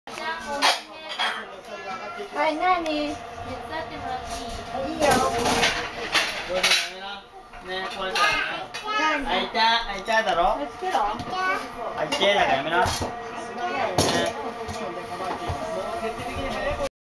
サッカー観戦in群馬 [2010.6.19]
Ｗ杯南アフリカ大会 日本-オランダ戦をパパの実家でじいちゃんに抱かれながら観戦。いとこの丈くんの方が気になる？